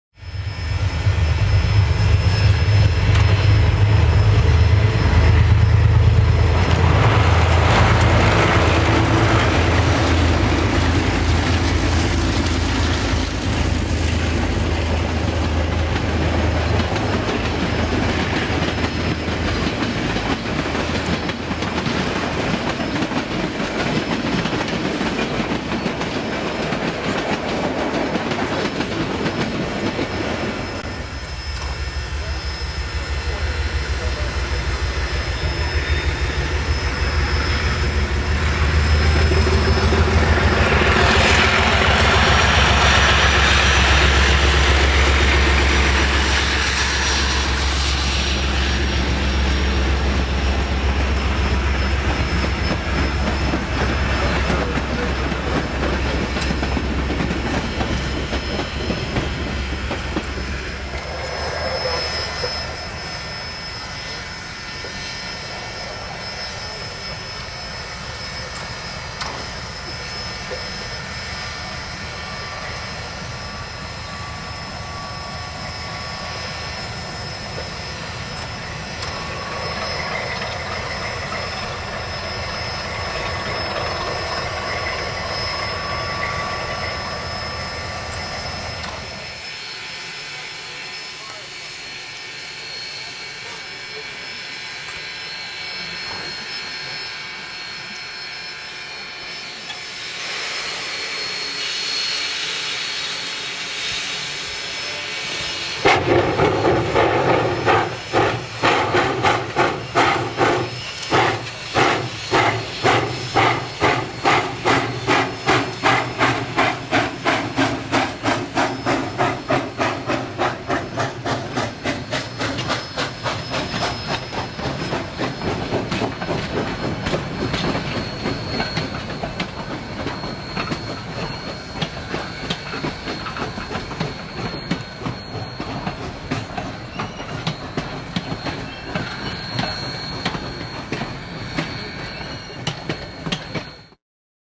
WP&Y Vignettes #22
These Vignettes of the White Pass and Yukon Ry. were shot from June 9 through June 13, 2011 on a special photo excursion on the railway.